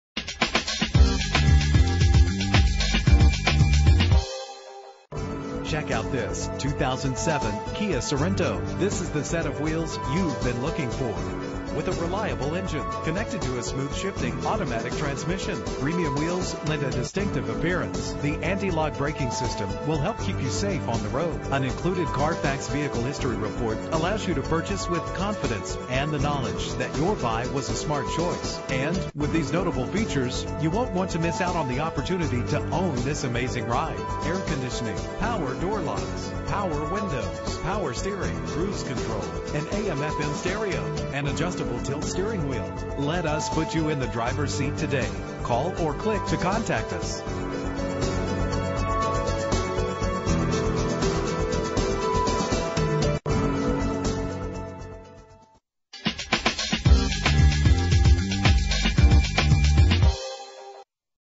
2007 Kia Sorento - MESA AZ
Year: 2007
Make: Kia
Model: Sorento
Trim: LX 4D SUV Sport Utility
Engine: 3.8 Liter
Transmission: Automatic
Color: Blue
Mileage: 37208
Address:
6136 E AUTO LOOP AVE